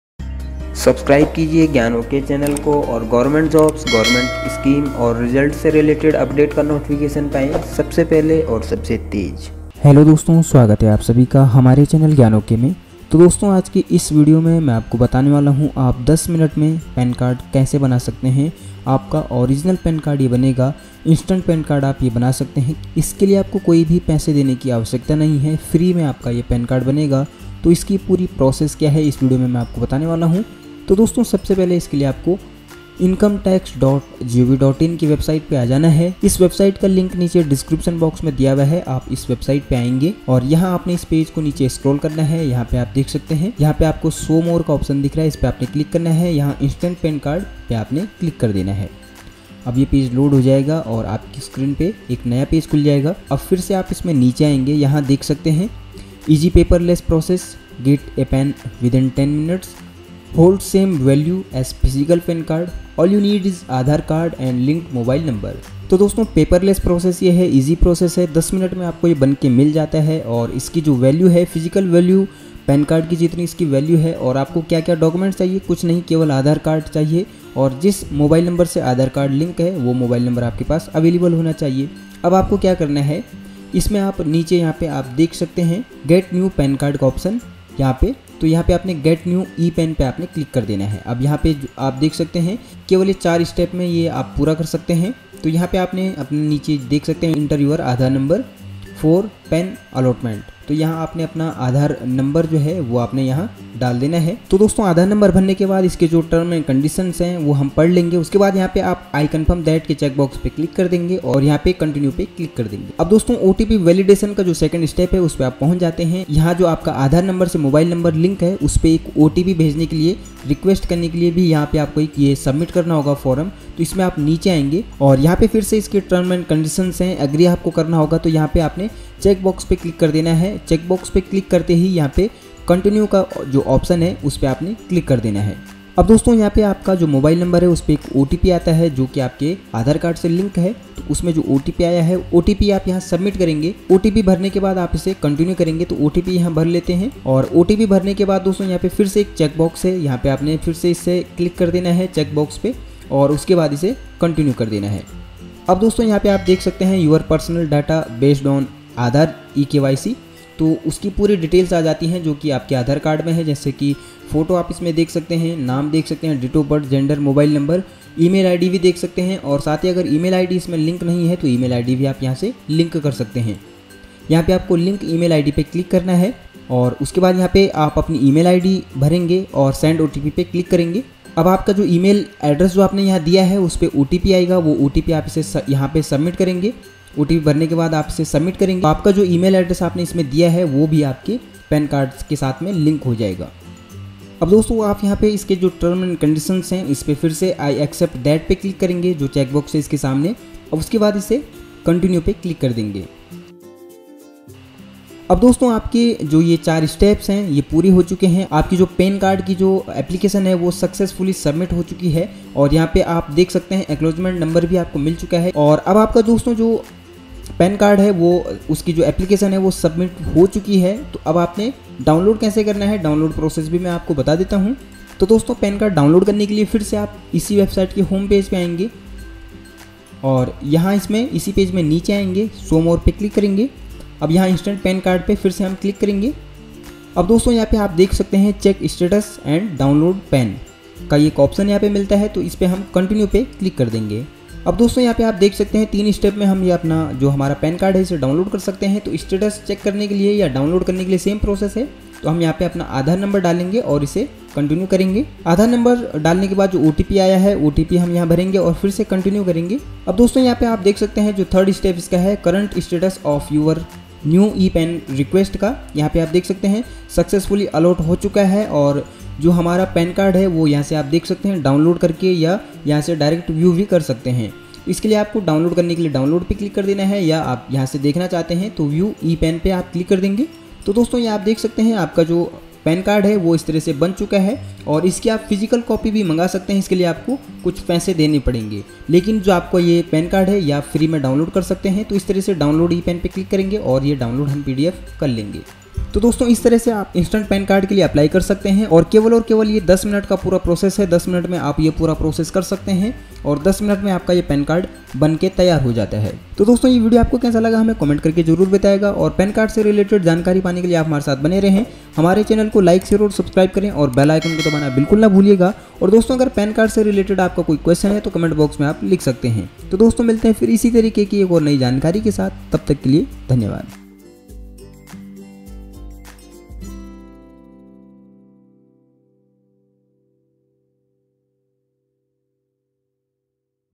instant pan card apply online - e pan card kaise banaye | pan card download | income tax new portal
Income tax new portal launch ho gya hai. jisse instant pan card ya e pan card banane ka process bhi change ho gya hai. es video me instant pan card banane ka complete process dikhaya gya hai aur ye bhi ki apply karne ke baad pan card download kaise kare.